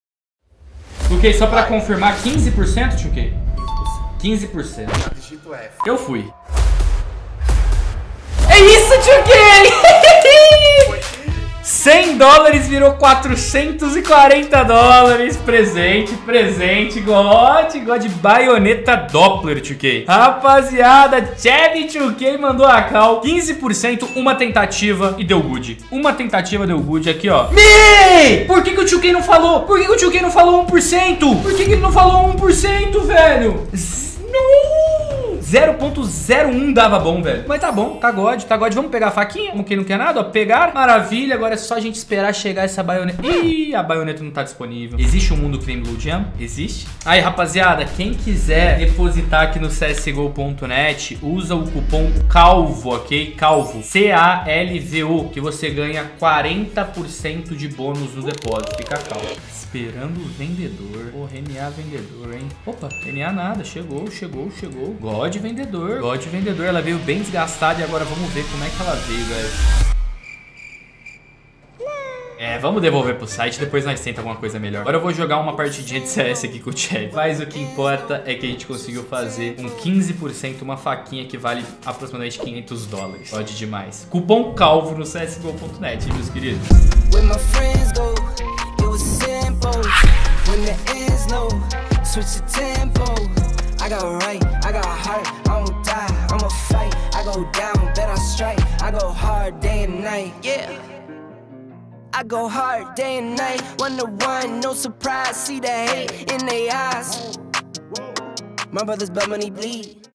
Sheviii2k me deu a CALL e eu ganhei uma FACA no CSGO
O Sheviii falou que eu ganharia essa faca no CSGO se fosse com 15%, e eu ganhei essa FACA no CSGONet